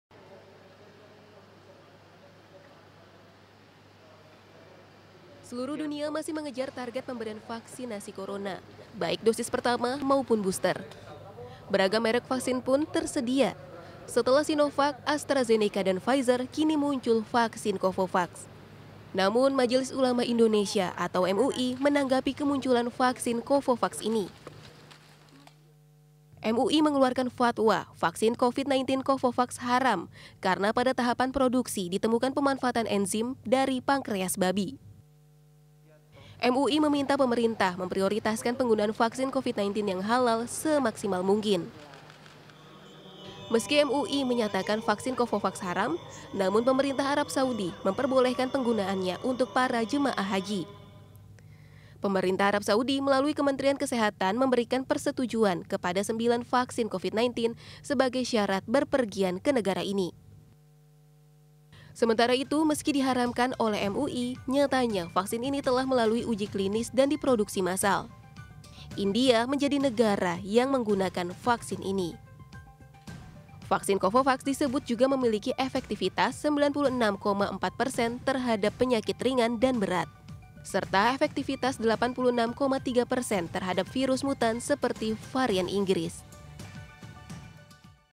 Vaksin Covid-19 Covovax Diharamkan MUI. Kenapa ya?
Majelis Ulama Indonesia (MUI) mengeluarkan fatwa vaksin Covid-19 Covovax asal India haram. MUI menetapkan fatwa haram karena pada tahapan produksi vaksin Covovax ditemukan ada pemanfaatan enzim yang diduga berasal dari pankreas babi.